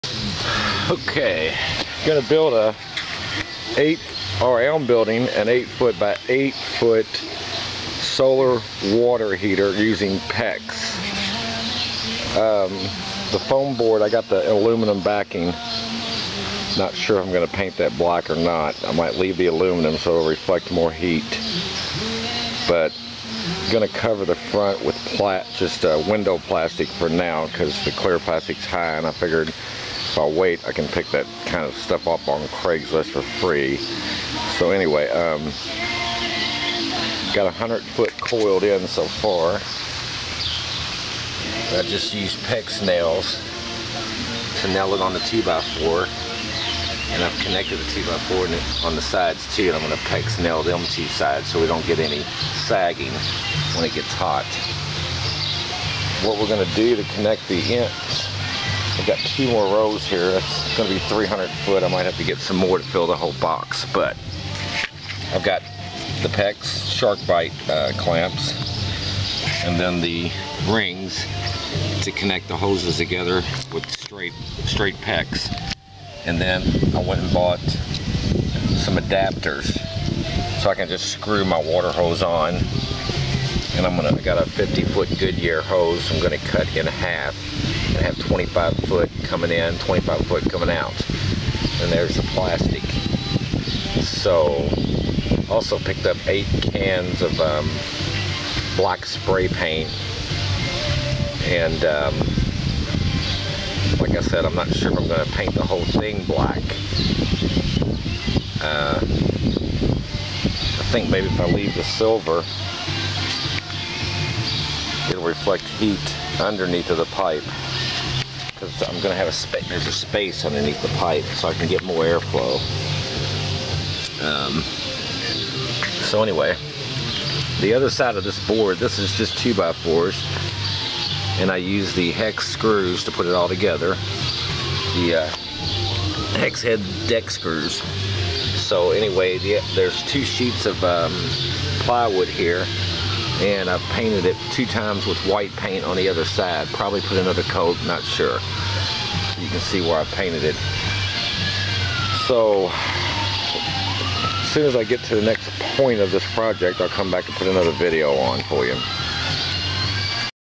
SOLAR HOT WATER TUBING/CAN POOL HEATER BUILD PART 1 of 5 8X8
This is part 1 of 5 videos that I took of me building an 8ft by 8ft Solar Water Heater for my pool using 216 drilled aluminum cans, two 4x8 sheets of OSB, two 4x8 sheets of silver back poly board, six 8 ft 2x4's boards, four 8 ft 2x2's, 20 cans of black flat spray paint,  470 ft of Pex Line and brass Pex fittins/Pex nails. The end result will show the full build with four sheets of 2x8 Plastic Lexan attached and sealed on the edges with formed rubber. I attached the lumber together using hex deck screws. The entire box is sealed with 100% silicon. The in and out hose fittings are plastic because the chlorine in the pool was deteriorating the brass fittings. I also spray painted the hoses black. I used a small low flow centrifugal force fountain pump for the water flow. The water coming out of the hose is around 110 degrees on an 80 degree day and around 150 degrees in the box. The box is connected to post in the ground using nuts and bolts so I can rotate the box at different angles during the day, depending where the sun is in the sky.  Hope you like my videos and share them as well. Any questions just ask. Thank you.